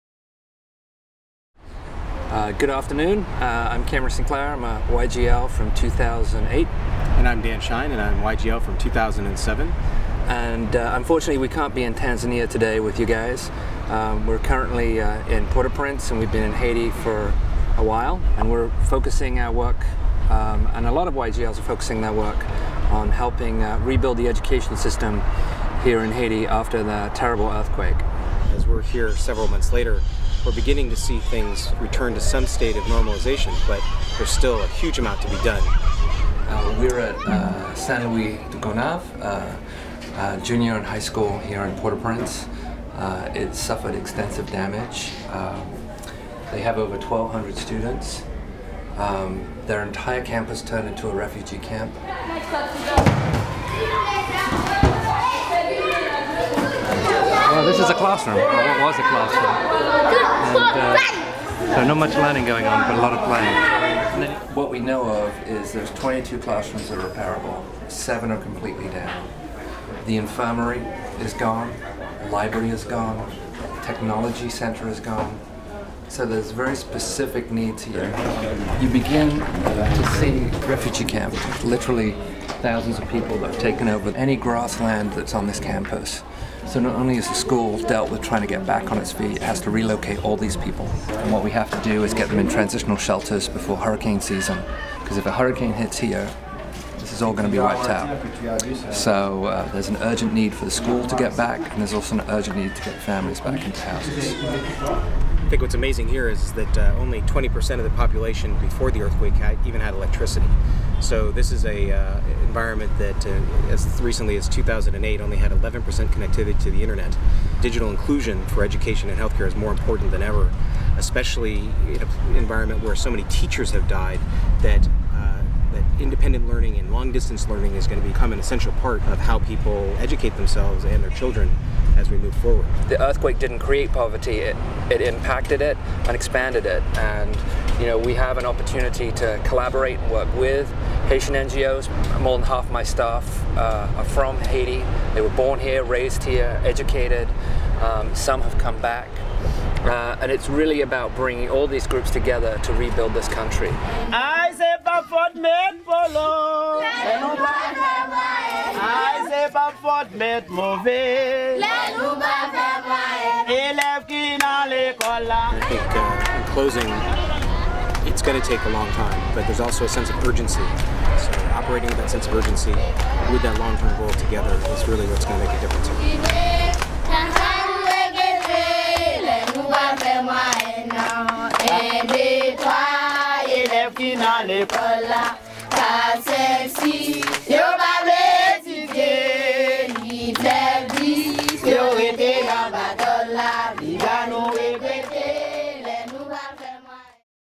Message from Haiti
Message from Haiti to Young Global Leaders summit in Tanzania from Cameron Sinclair of Architecture for Humanity and Dan Shine of 50x15 Foundation, shot by Turk Pipkin during AfH/Nobelity Project and 50x15 site inspections for school reconstruction projects.